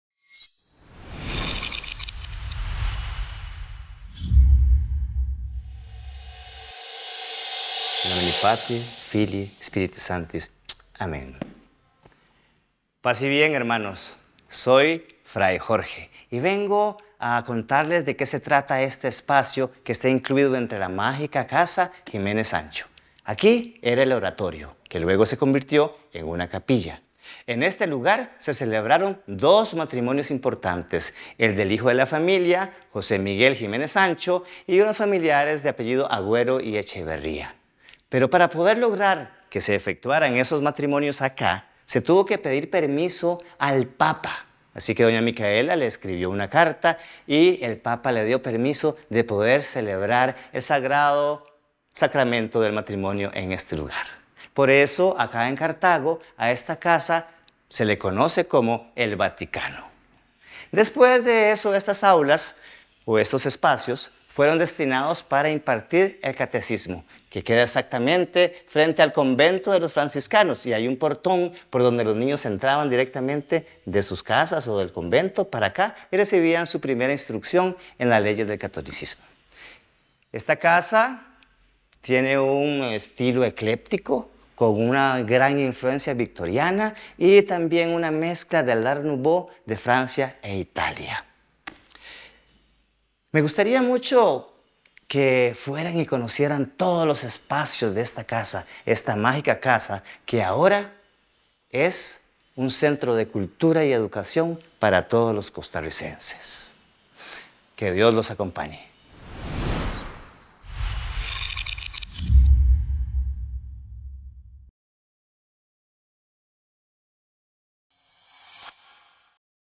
Recorrido Casa Jiménez Sancho – Capilla
El Fraile Jorge nos comparte la inusual historia detrás del apodo de “El Vaticano” que ostenta la Casa Jiménez Sancho en la ciudad de Cartago.
Los personajes interpretados por los actores son ficticios, el contenido histórico obedece a hechos reales. Para conocer más de la Casa Jiménez Sancho le invitamos a visitar nuestro sitio

Fraile Jorge: José Chaves
Producción y Dirección de Fotografía: José Miguel González
Asistencia de Producción: Rodrigo David Gutiérrez y Raquel Zúñiga
Dirección Escénica y Guion: Uriel Morera
Especialistas de Contenido: Rodrigo David Gutiérrez, Natalie Quirós y Lucía Arce
Operador de Cámara A: José Miguel González
Operador de Cámara B: José Manuel Meneses
Asistencia de Cámara: Andrés Alvarado
Iluminotécnico: Santiago Martínez
Sonido Directo: Gabriela Rivas
Maquillaje: Viviana Ruiz
Vestuario: Uriel Morera
Edición y Corrección de Color: José Miguel González
Animación y VFX: José Mario Quesada
Diseño Gráfico: Raquel Badilla
Administrador de Producción PPMA: Rodrigo Ureña
Encargada de Presupuesto: Evelyn Pérez
Encargado de Plataformas: Minor López
Coordinación PPMA: Luis Fernando Fallas

Agradecimientos:
Laura Vargas, Vicerrectora Ejecutiva, administradora a.i. Casa Jiménez Sancho
Guiselle Blanco, Coordinadora Programa de Promoción Cultural
Alfonso Solórzano, Jordi Chavarría y Eilyn Agüero, Personal de la Casa Jiménez Sancho
Susan Solís y Natalie Quirós, Cátedra de Turismo Sostenible.
Unidad de Transportes, UNED